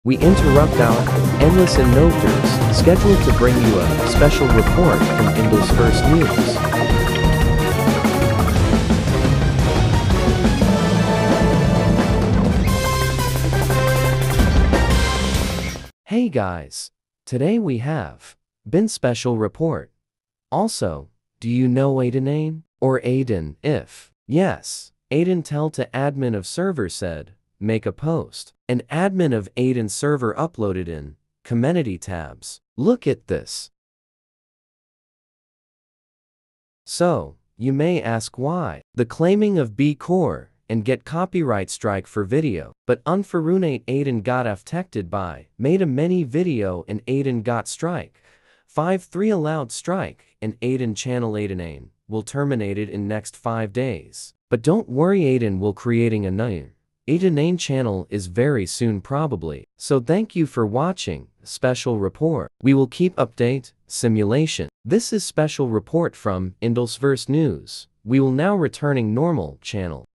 SPECIAL NEWS REPORT NOW! (Read desc for who is he)
What happened to Ayden (Aydenane) will let’s find out
He is core game and he have 9.40K+ subs or more

You need to watch this RIGHT NOW! ​⁠@zoopla2323 ​⁠@Thunderjoon_HongkongMacau ​⁠@theyellow2285 ​⁠@davidelijah_not ​⁠@BinTech2006 ​⁠@NewFall and more Core game
Forget to mention: @MCR_Roblox